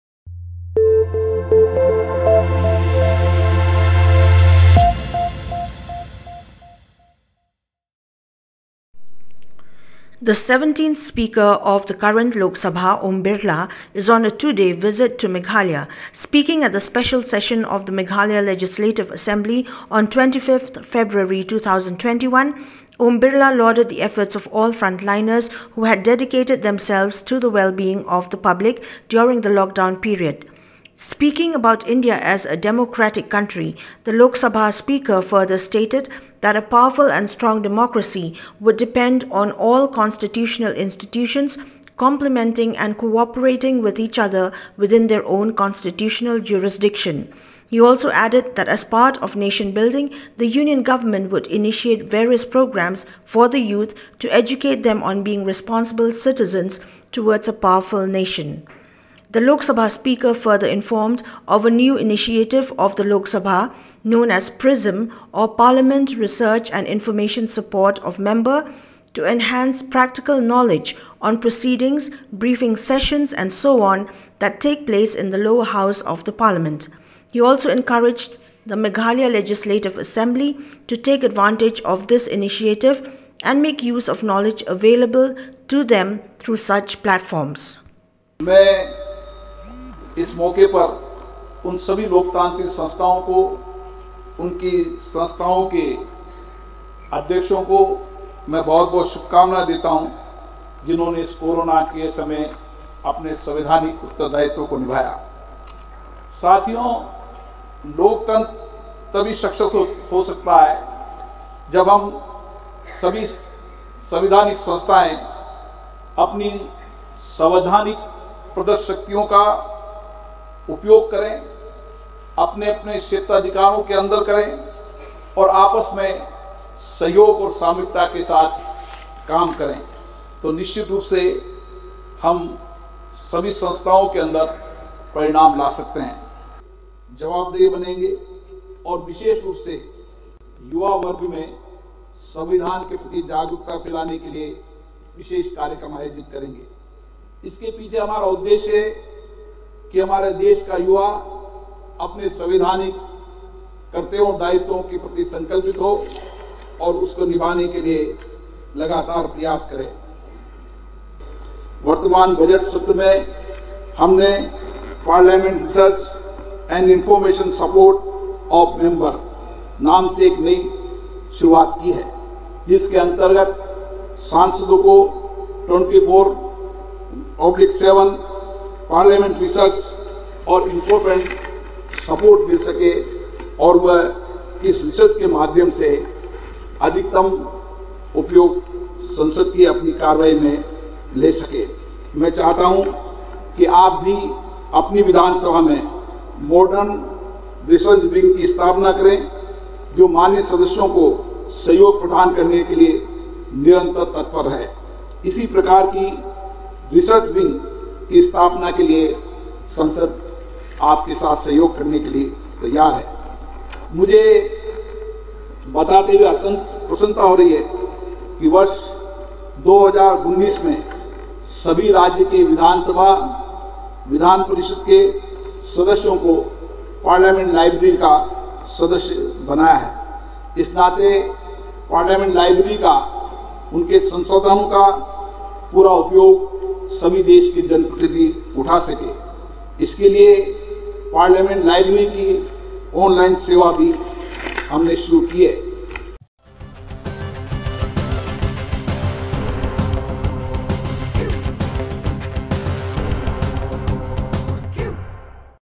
LOK SABHA SPEAKER OM BIRLA ADDRESSES MEMBERS OF THE MEGHALAYA LEGISLATIVE ASSEMBLY
THE 17TH SPEAKER OF THE CURRENT LOK SABHA, OM BIRLA IS ON A TWO DAY VISIT TO MEGHALAYA.  SPEAKING AT THE SPECIAL SESSION OF THE MEGHALAYA LEGISLATIVE ASSEMBLY, OM BIRLA LAUDED THE EFFORTS OF ALL FRONT-LINERS WHO HAD DEDICATED THEMSELVES TO THE WELL BEING OF THE PUBLIC DURING THE LOCKDOWN PERIOD. SPEAKING ABOUT INDIA AS A DEMOCRATIC COUNTRY,  THE LOK SABHA SPEAKER FURTHER STATED THAT  A POWERFUL AND STRONG DEMOCRACY WOULD DEPEND ON ALL CONSTITUTIONAL INSTITUTIONS COMPLEMENTING AND COOPERATING WITH EACH OTHER WITHIN THEIR OWN CONSTITUTIONAL JURISDICTION.
HE ALSO ADDED THAT AS PART OF NATION BUILDING, THE UNION GOVERNMENT WOULD INITIATE VARIOUS PROGRAMS FOR THE YOUTH  TO EDUCATE THEM ON BEING RESPONSIBLE CITIZENS TOWARDS A POWERFUL NATION.